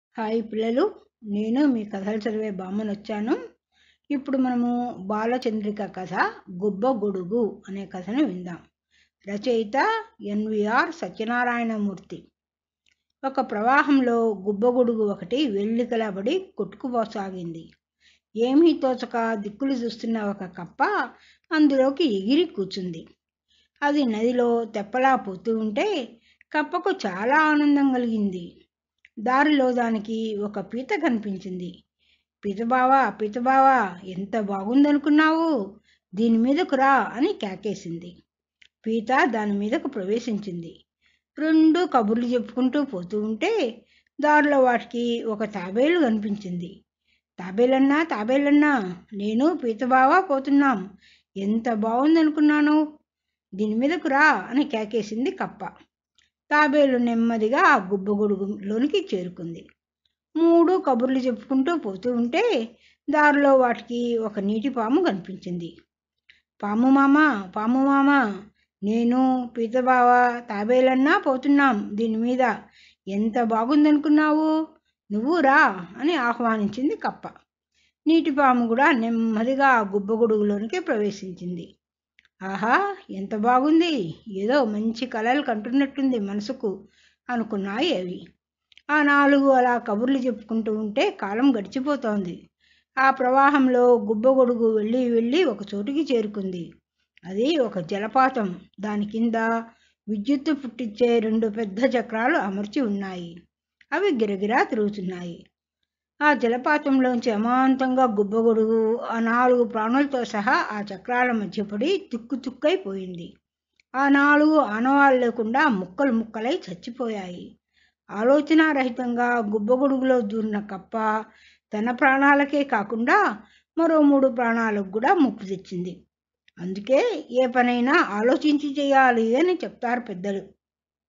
Gubbagodugu Balachandrika kadhalu
A short story from Balachandrika magazine

Let me know in comments if any improvements are required